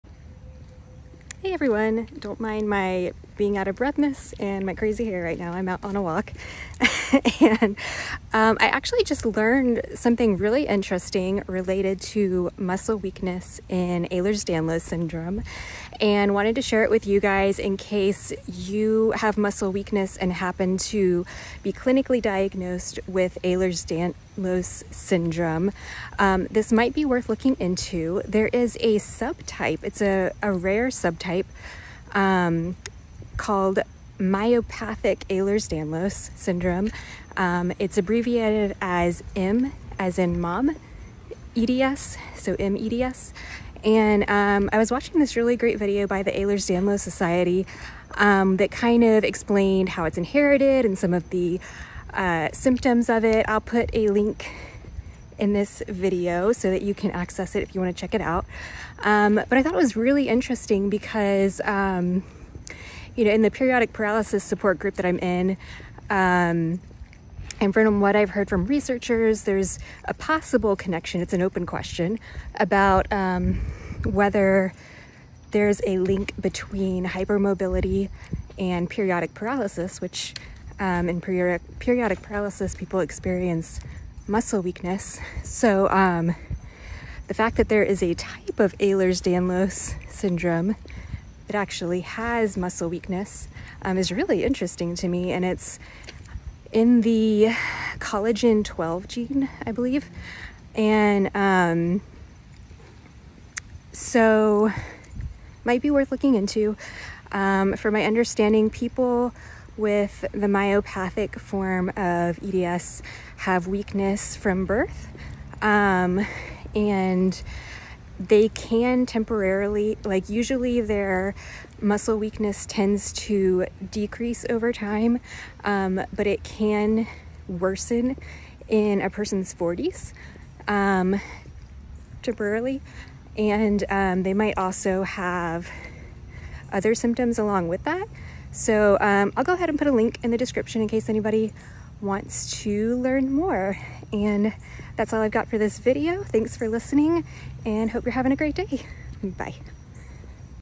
Myopathic Ehlers Danlos Syndrome (mEDS)
Did you know there's a rare type of Ehlers Danlos Syndrome that can cause muscle weakness? If you think you may have EDS, have been clinically diagnosed with EDS, and/or you experience muscle weakness, it might be working looking into myopathic EDS.

Here's a link to the Ehlers Danlos Society video mentioned in the video: